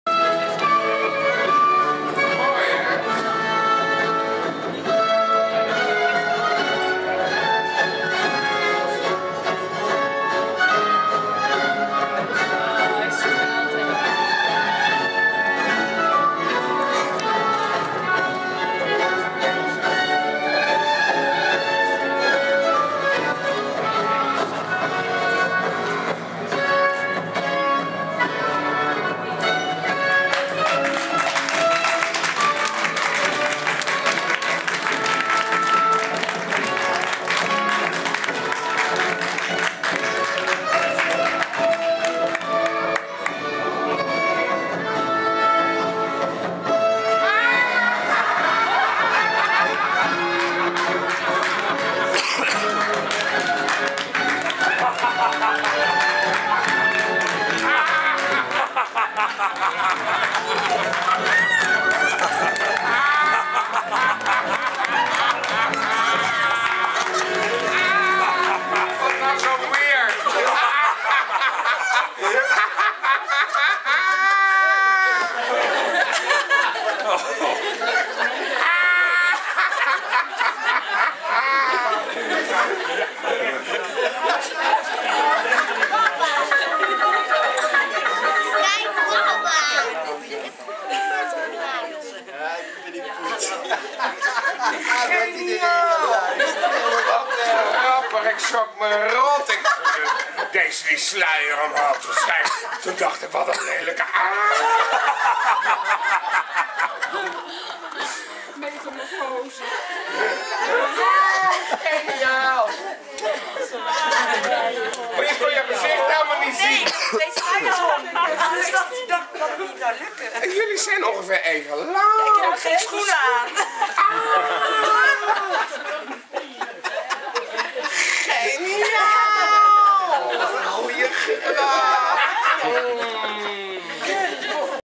Frank Los beetgenomen door zijn aanstaande vrouw.
Wie wil dit nou niet, twee bruiden.
Toch is ervoor Frank Los maar één echte bruid.